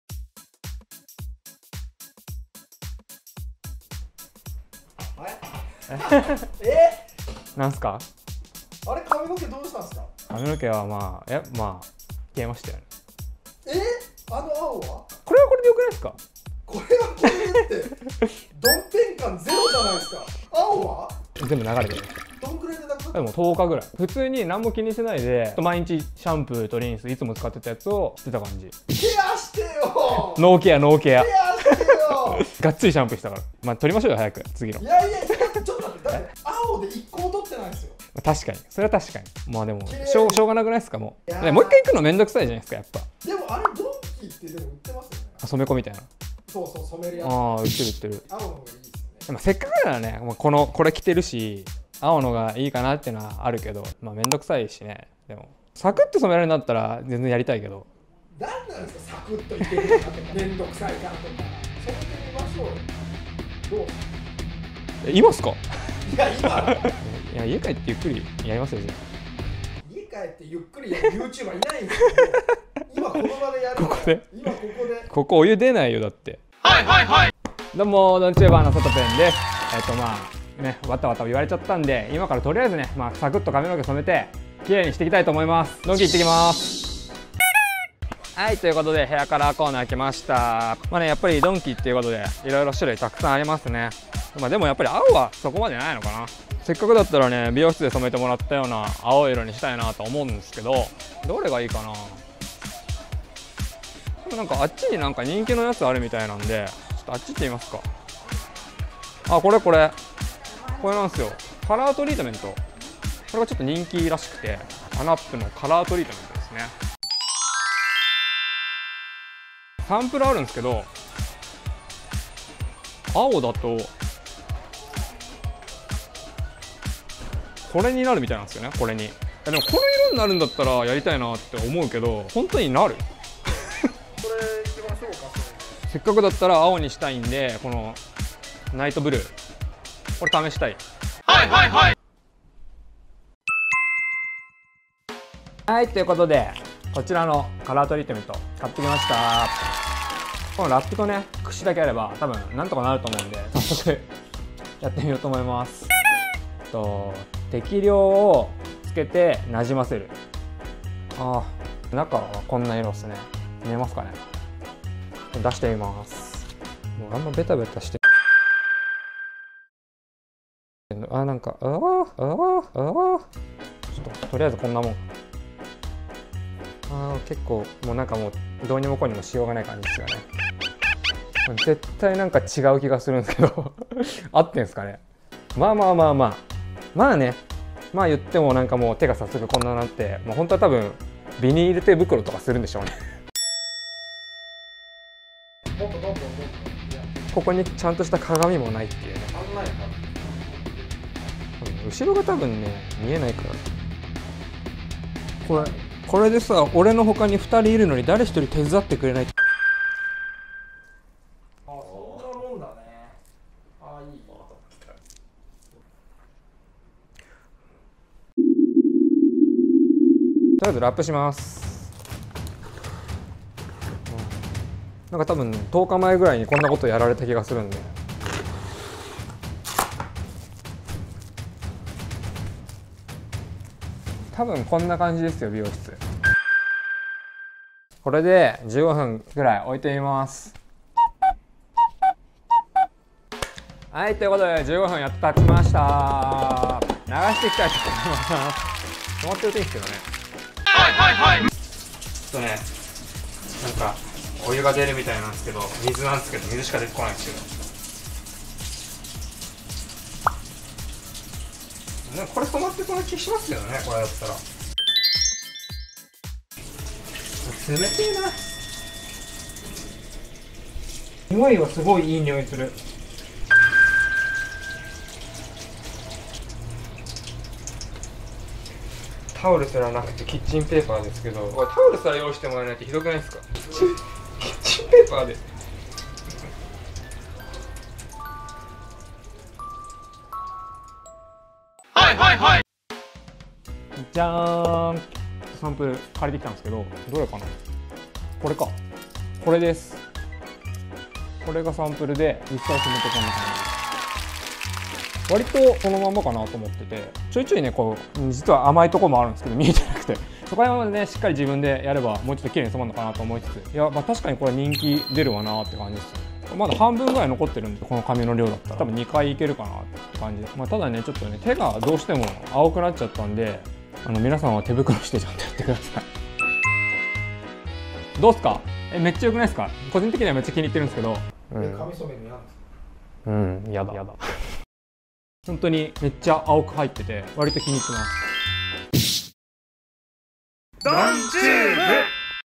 【また染める】青って色抜けやすいんよ。。。【ドンキ公式】
DonTube＝ドン・キホーテ公式YouTubeチャンネル

第5回目の配信は、「青って色抜けやすいんよ。。。」

前回の配信で、認知度を改善させるべく思い切って青髪に！！
ところが、あれ、どうした、え！？って感じになっちゃいまして・・・笑

まぁ結論、青髪気に入ってるんです！

そろそろ本題行けよって思ってる皆さ〜ん
安心してください。次の動画は髪染める動画ではないです笑

byさとぺん